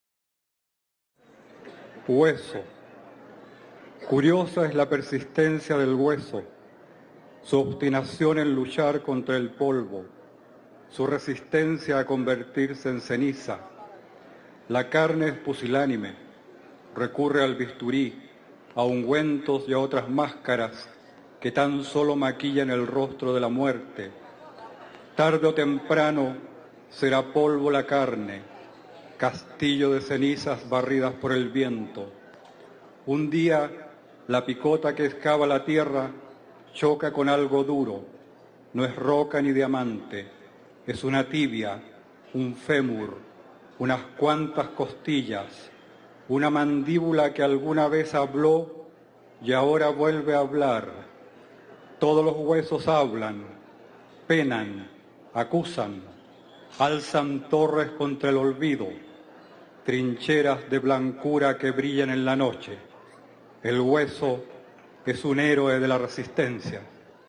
Poeta Óscar Hahn - Hueso.
Lectura del poema Hueso en: Festival Internacional de Poesía de Medellín.

Poeta Chileno nacido en Iquique, Óscar Hahn, es autor de libros como: Esta Rosa Negra, Mal de Amor, Arte de Morir y su poética es bastante versátil y se nutre de múltiples géneros y voces.